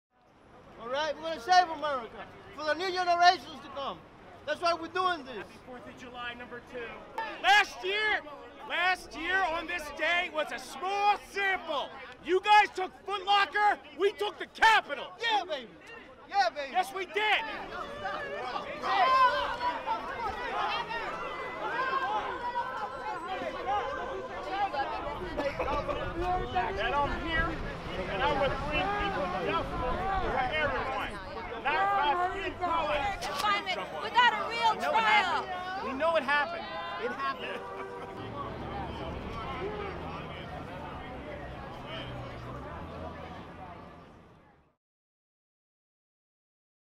وقفة احتجاجية لأنصار ترمب في نيويورك في ذكرى أحداث الكابيتول
أقيمت وقفة احتجاجية من أجل "إنقاذ الديمقراطية" في أبر ويست في نيويورك في الذكرى الأولى لأحداث الشغب في الكابيتول في 6 يناير. احتفل عدد قليل من أنصار دونالد ترامب بهذا اليوم أمام كاتدرائية القديس باتريك في وسط المدينة